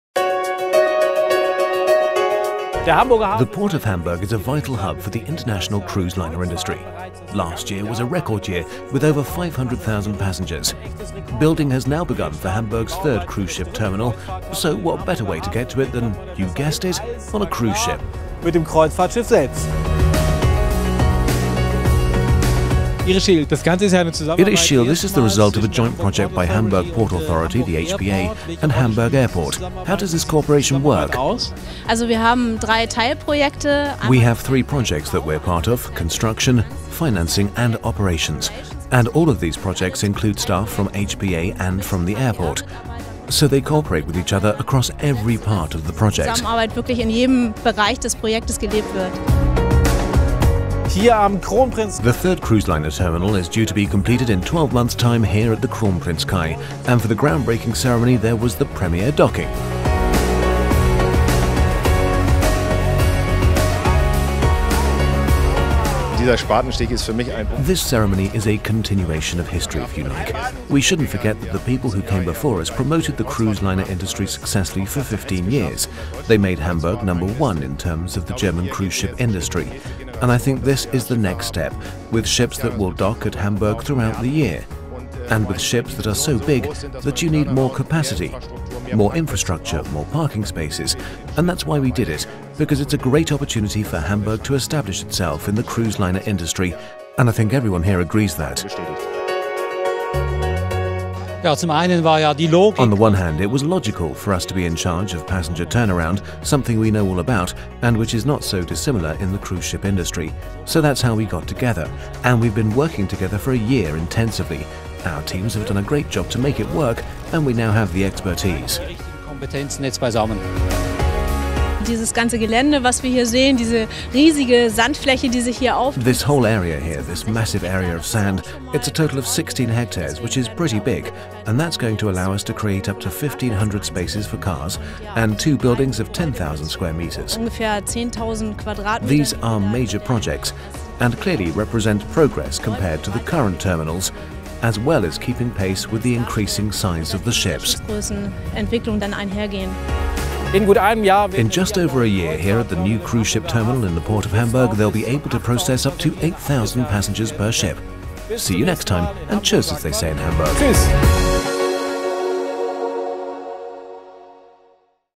Building of the Cruise Terminal No 3 in Hamburg [ENG]
Hamburg is one of the ten biggest cruise ports in Europe and number one in Germany. With the third cruise terminal in the port of Hamburg the cruise business will grow straight on. The new terminal will be able to handle 8.000 passengers per ship.